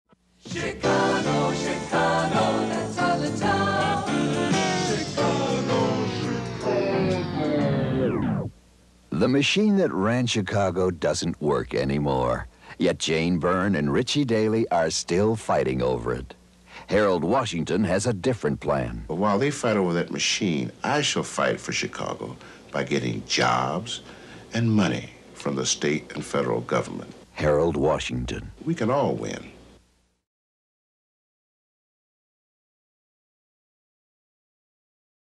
Harold Washington For Mayor of Chicago 1983
Harold Washington running for Mayor of Chicago in 1983.